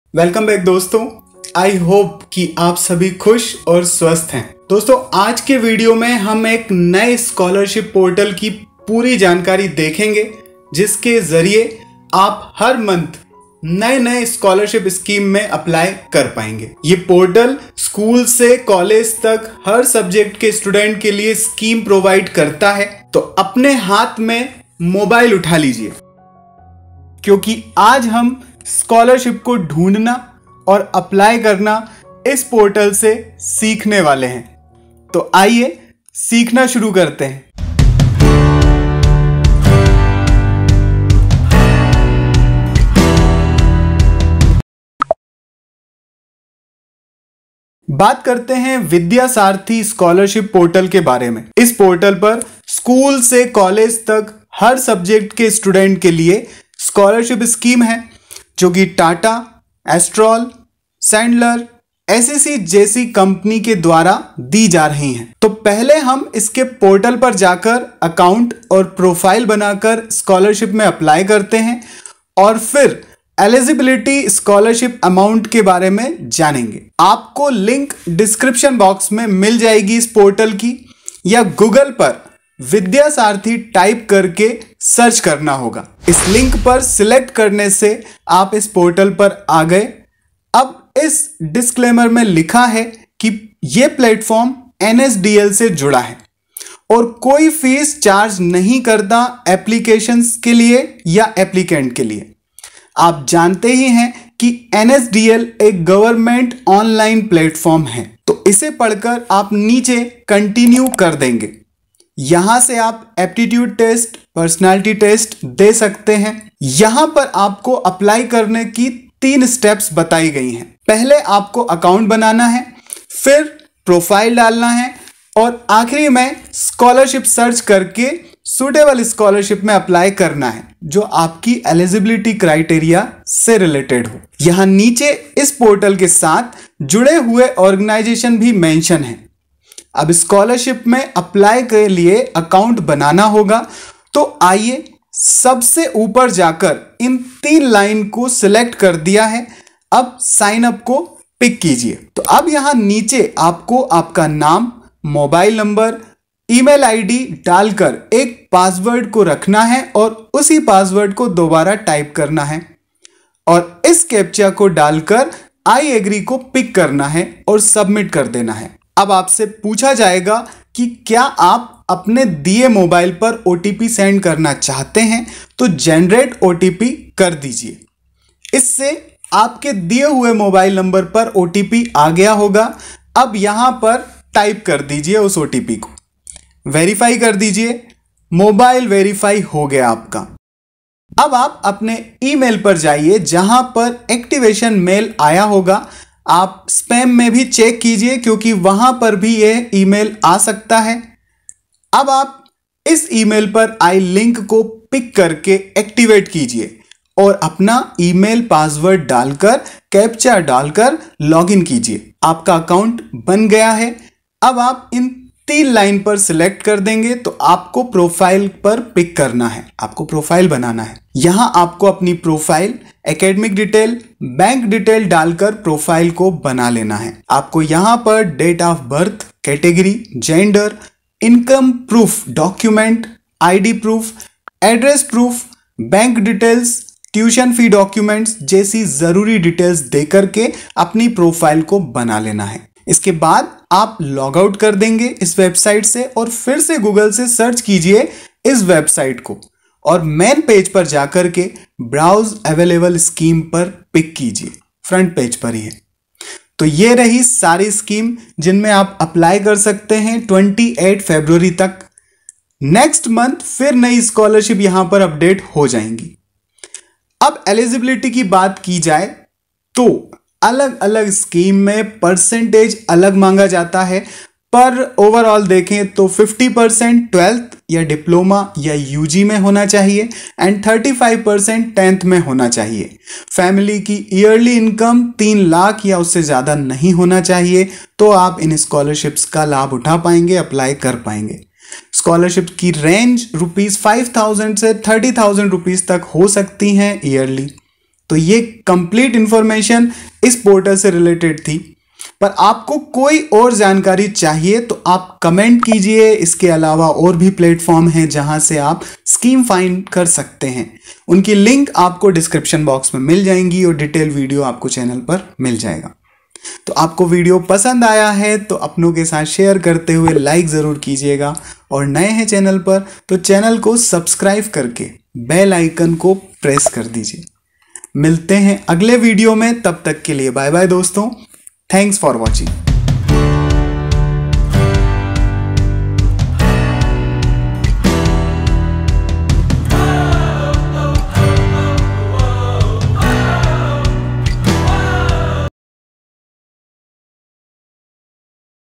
Vidyasaarathi Scholarship Portal – 2021 I Free Registration I Rs. 50,000 Amount
Hello Friends!!
By this video, you can got full details of Vidyasaarathi Scholarship Portal. Any Indian students (school & college) can apply for scholarship. Maximum amount is Rs. 50,000/- BE MBA BBA 10th 12th ITI Diploma BSc (Agriculture) Medical BEd etc. candidates eligible for this scholarships.

Percentage required - for 12th - 50% & 10th -35%
Family income upto Rs. 3,00,000/-.